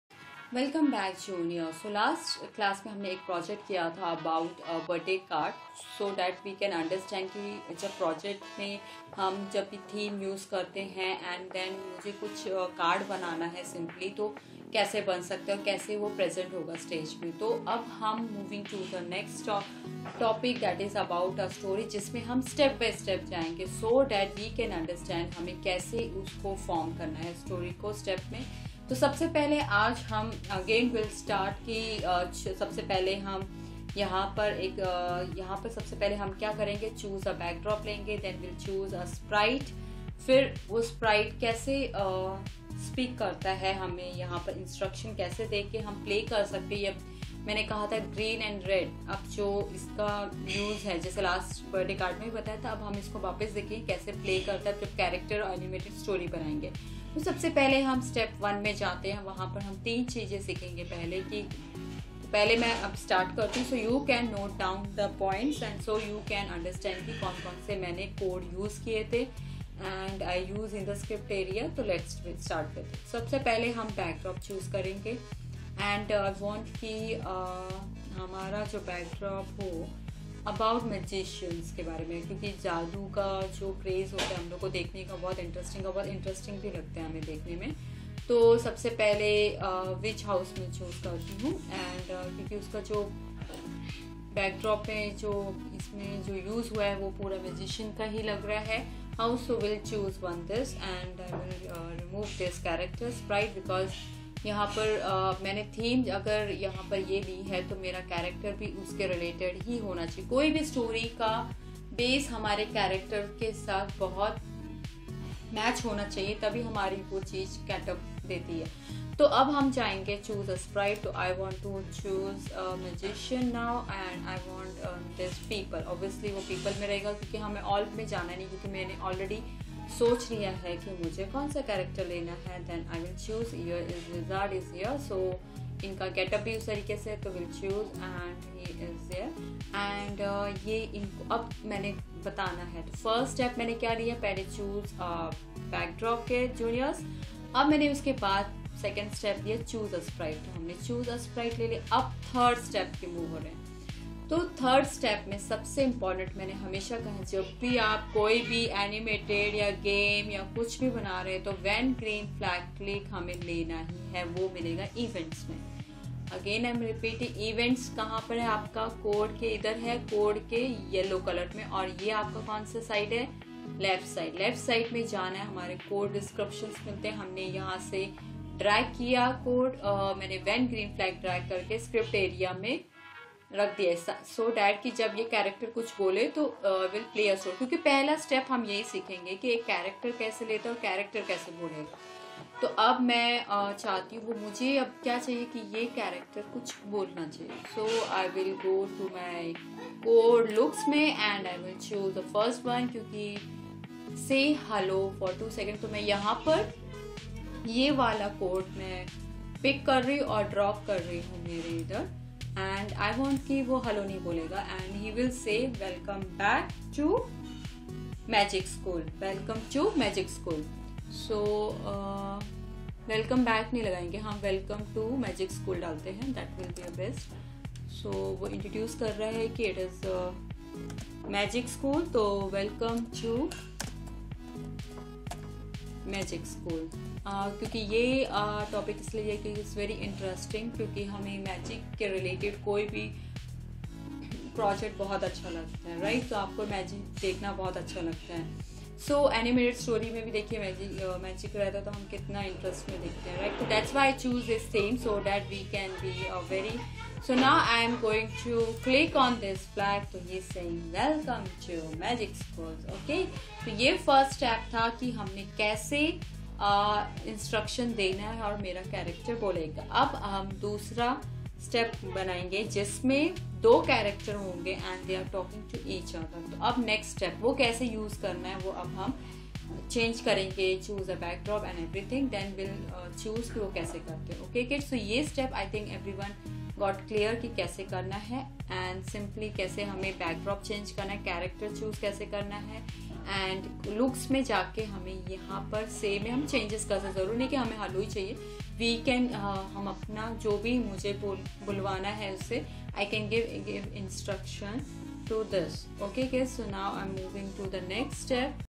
Scratch|Scratch tutorial in Hindi|Set the scene and have a character say something
In this video going|Scratch tutorial in Hindi|Choose a character, add conversations and bring story to life|To change the direction a character is facing, click the Costumes tab, then click Flip Horizontal|Make two characters talk to each other|ADD THIS CODE|Click the thumbnail for each character, and then add its code|Set the scene and have a character to say something|Choose a Witch Housebackdrop|Choose a character wizard|Click the green flag to start

Thanks & Regards,
Kids Learning